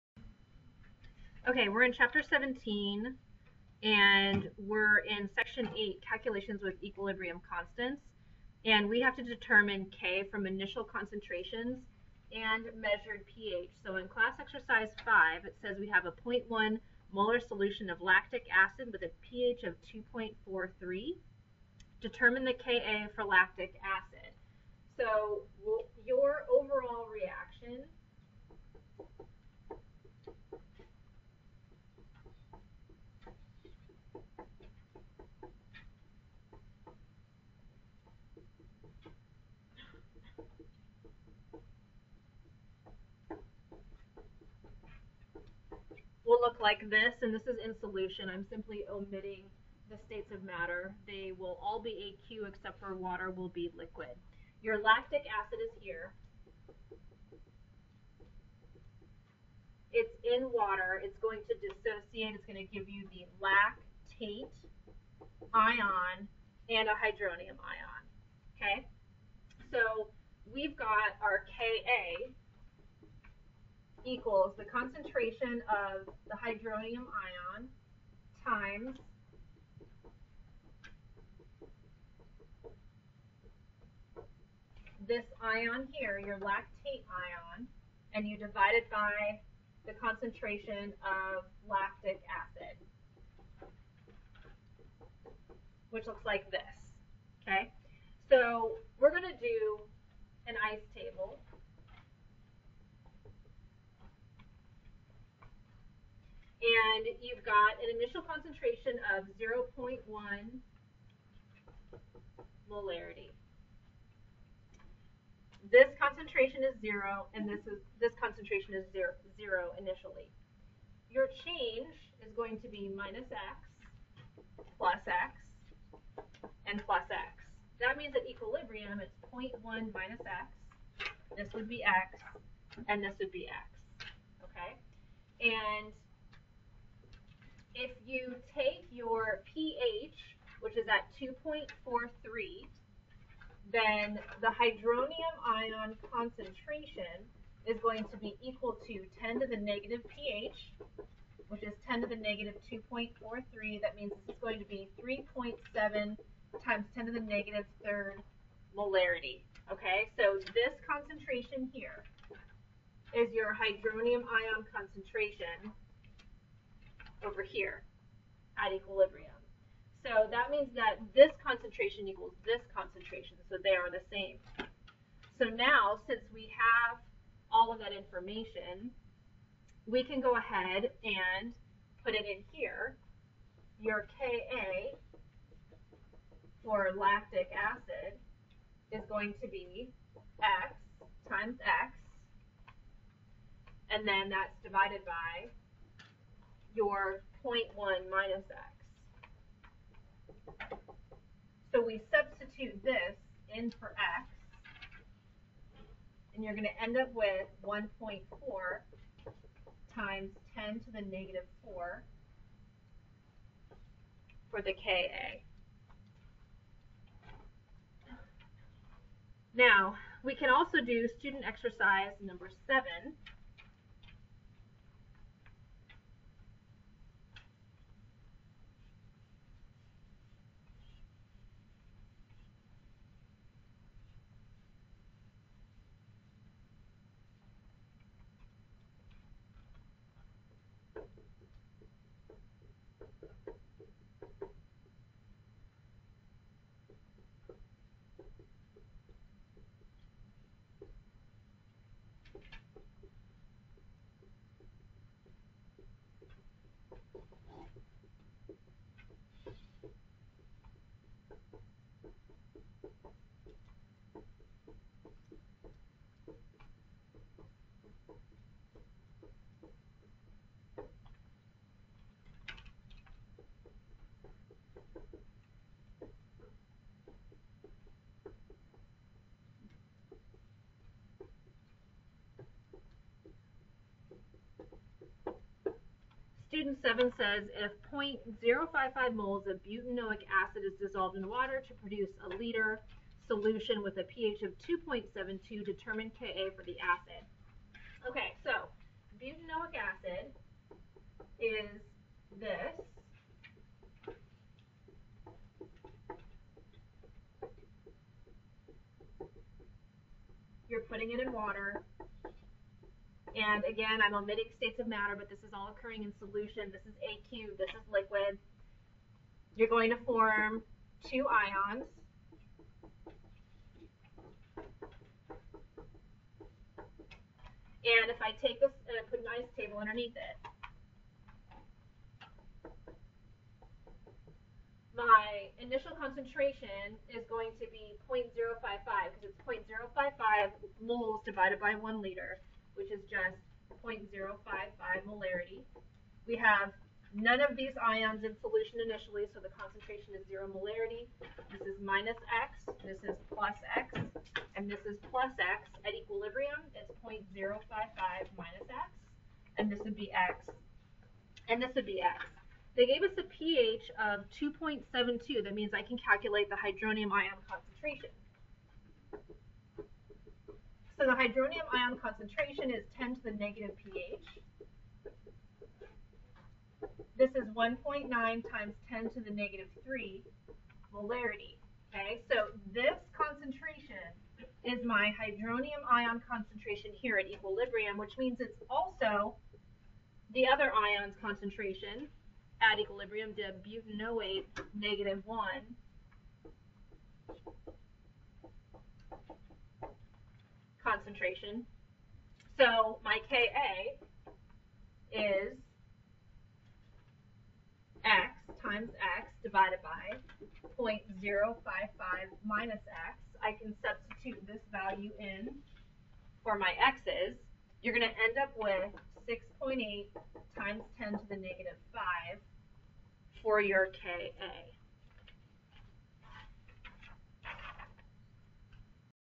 Equilibrium Constants: Determine K from Initial Concentration and pH
Calculations with equilibrium constants:  determining K from initial concentrations and pH.